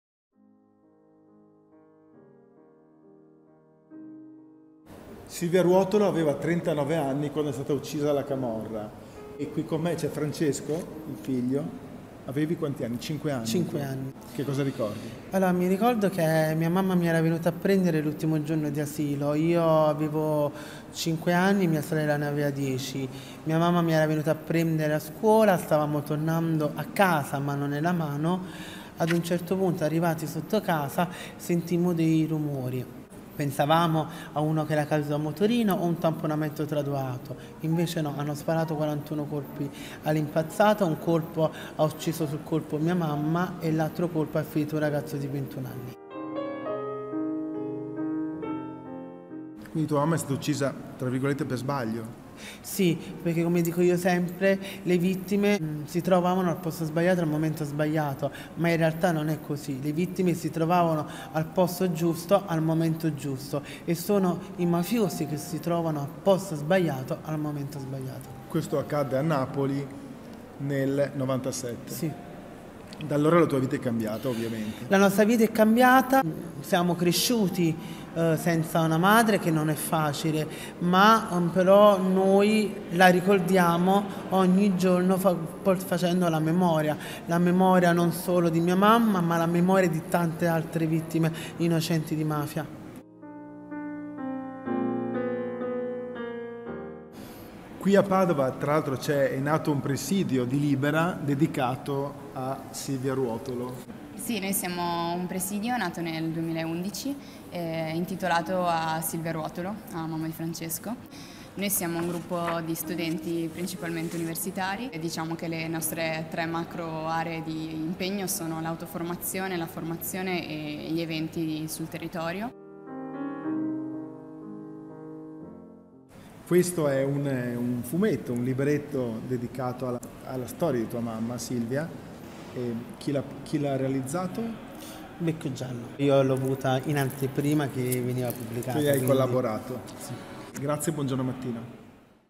Padova, Francesco racconta della mamma Silvia Ruotolo uccisa dalla camorra
"Hanno ucciso mia mamma quando è venuta a prendermi all'asilo": un figlio che ricorda, un fumetto che racconta e le iniziative di Libera Padova. Francesco Clemente e la memoria di Silvia Ruotolo nel buongiorno di Leandro Barsotti